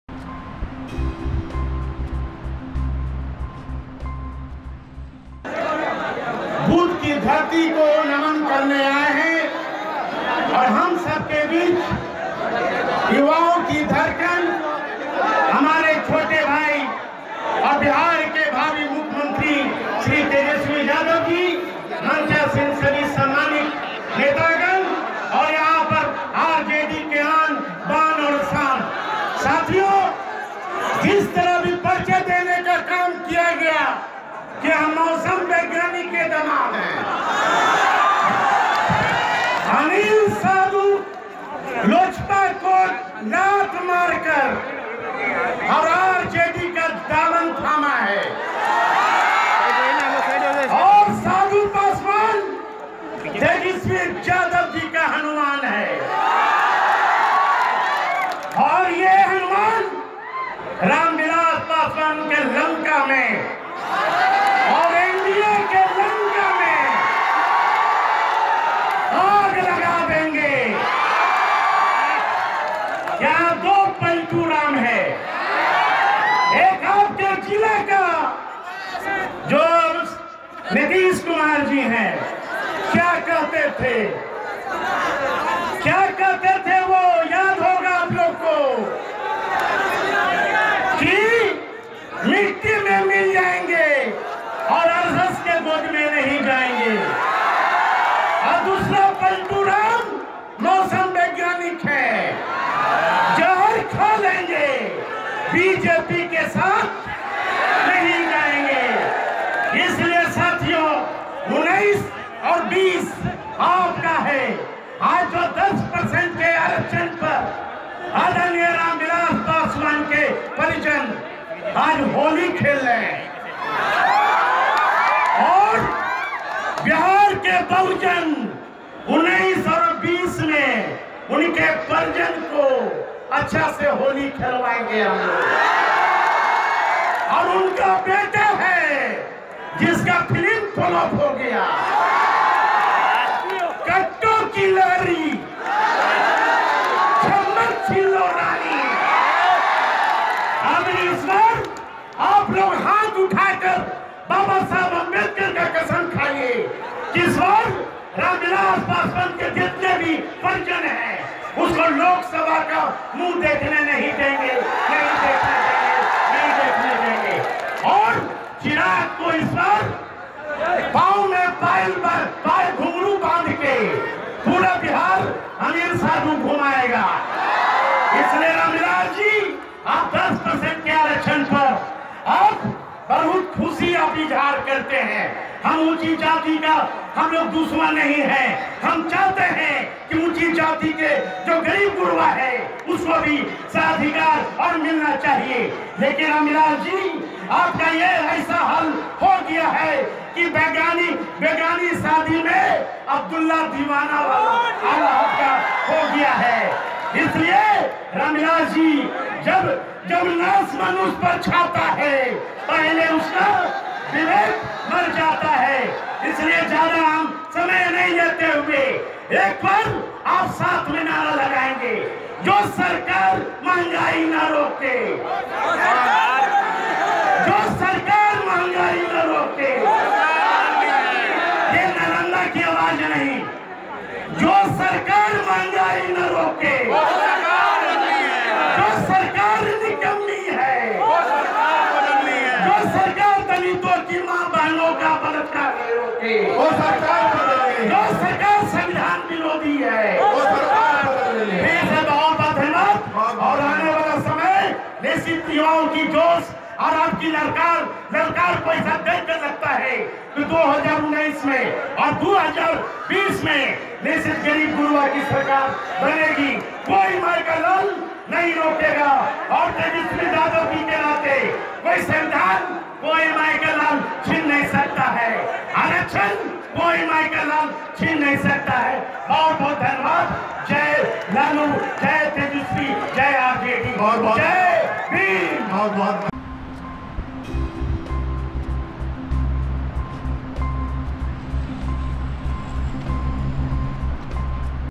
Ram Vilas Paswan पर बमक गए उनके दामाद Anil Kumar Sadhu, बोले मुंह नहीं देखने देंगे Loksabha का
Ram Vilas Paswan पर बमक गए उनके दामाद Anil Kumar Sadhu, बोले मुंह नहीं देखने देंगे Loksabha का l LiveCities